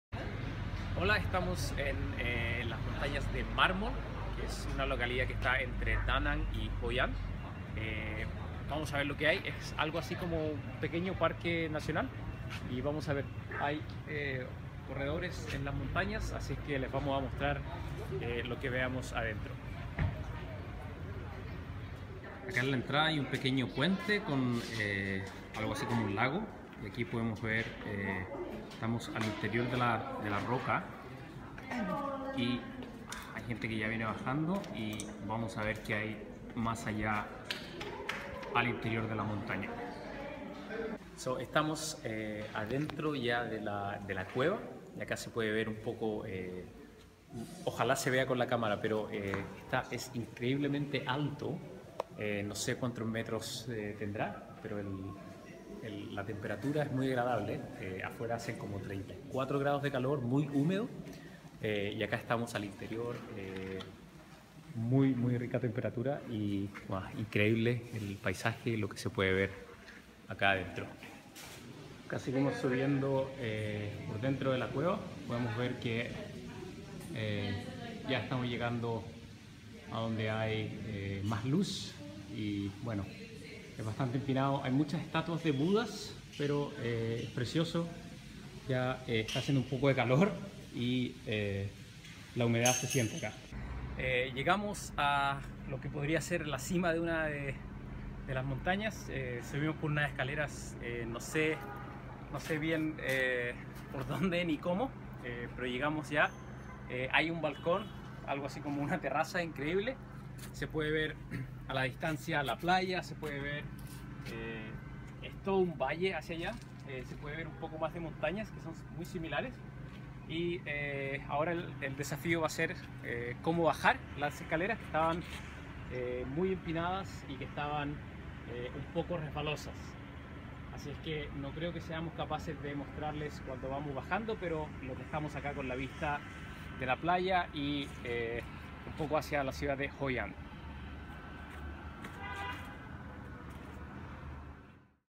Viaje a Vietnam - Hoi An - Montañas de Mármol
Daniel nos muestra una maravilla de la zona: las montañas de mármol.